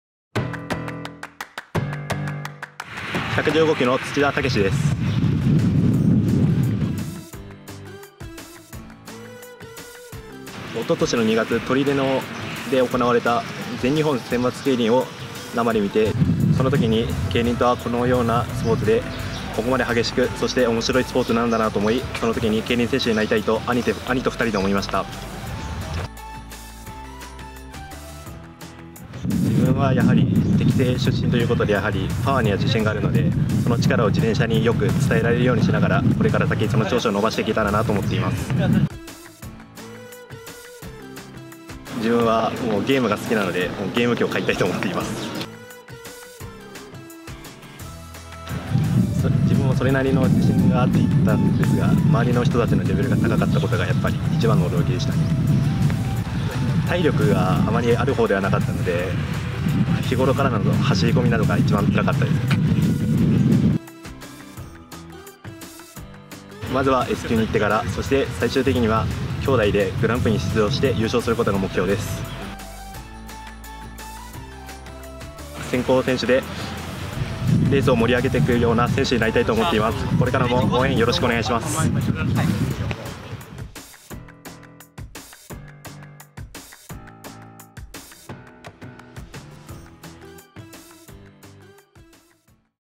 けいりんマルシェ 新人さんいらっしゃい！ 競輪 土田武志選手
2019年7月に競輪選手としてデビューする新人選手を紹介する、
けいりんマルシェ「新人さんいらっしゃい！」

今回は、茨城県の115期、土田武志(つちだ・たけし)選手。

競輪選手になったキッカケ、自分の長所、初めての賞金の使い道、選手としての目標などを質問し答えてもらいました。

◆けいりんマルシェ
記事を読んでポイント貯めると素敵なプレゼントをゲットできる！